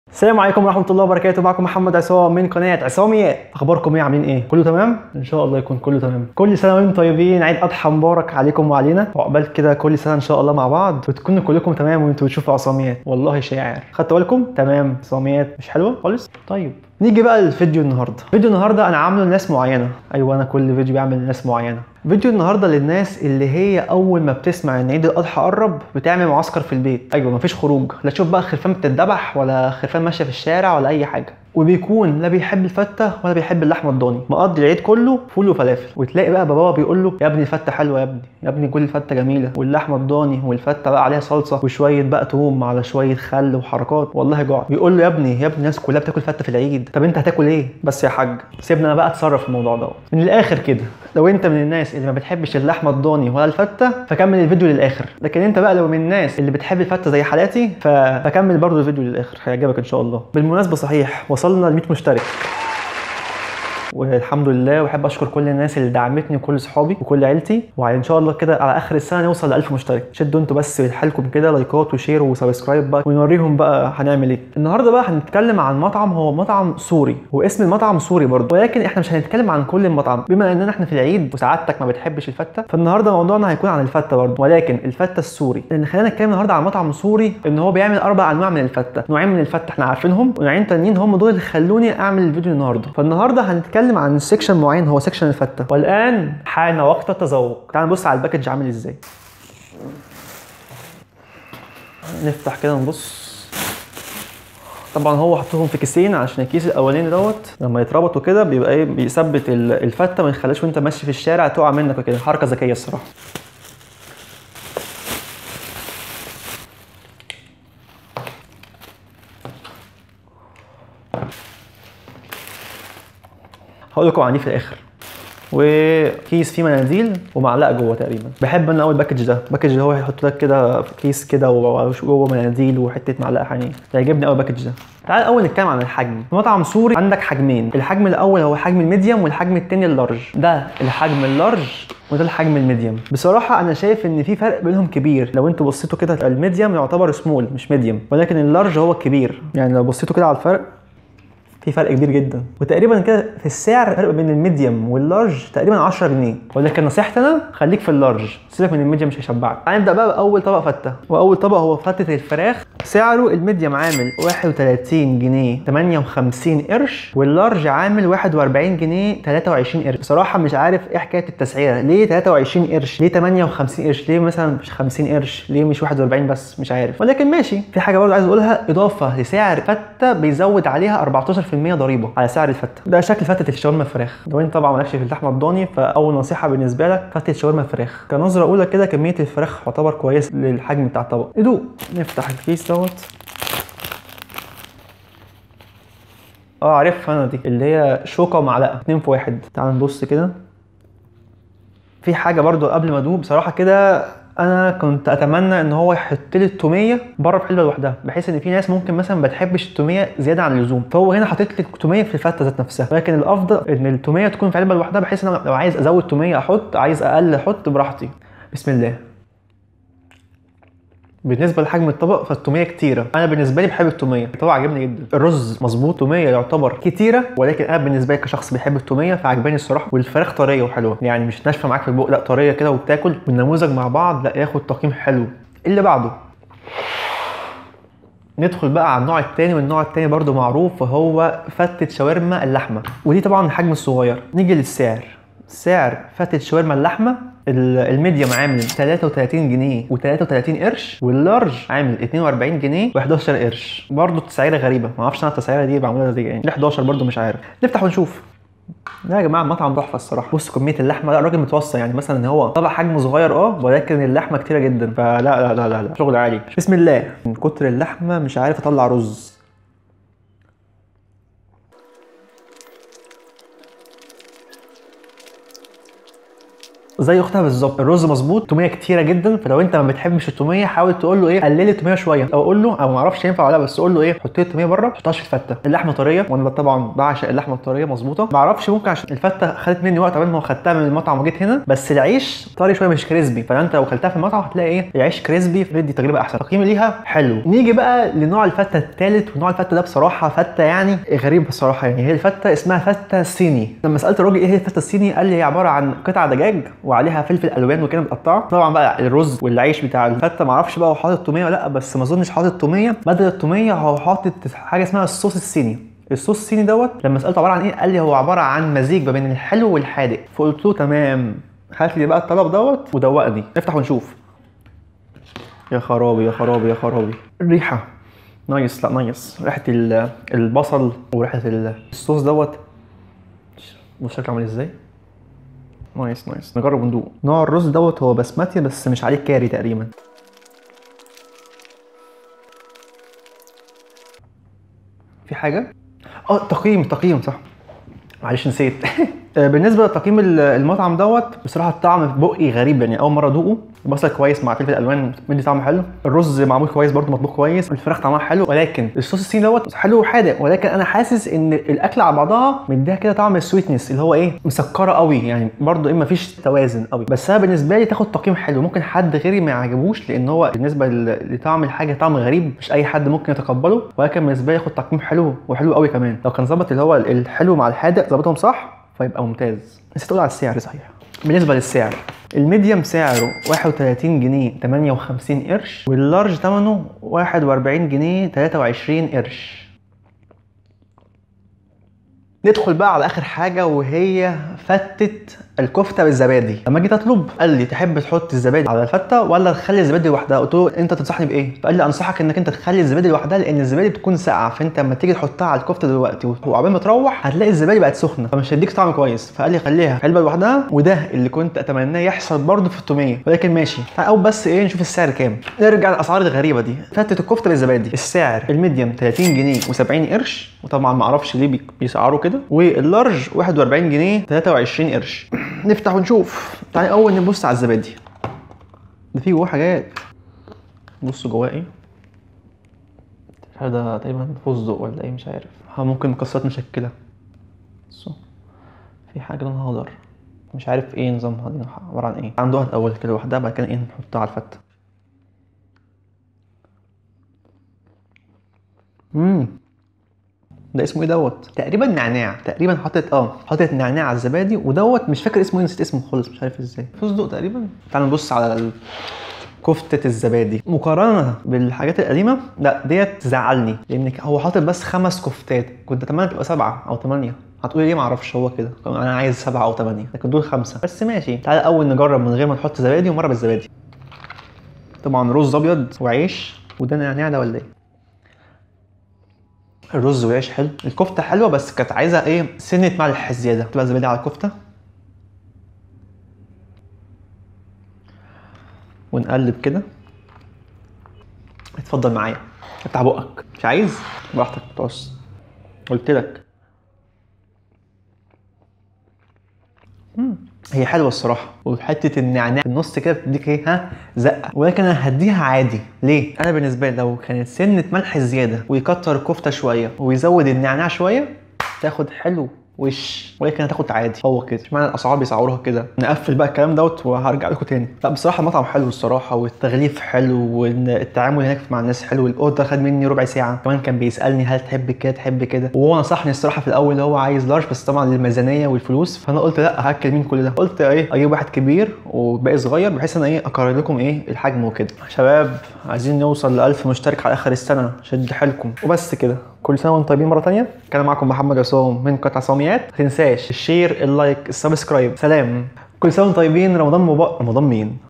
تجربة جميع انواع الفتة من مطعم سوري
كل سنة و انتم طيبين
عيد اضحي مبارك عليكم
فيديو النهاردة للناس اللي مش بتحب الفتة و لحمة الخروف الضاني
هنجرب كذة نوع فتة من مطعم سوري
يارب الفيديو يعجبكم

(105)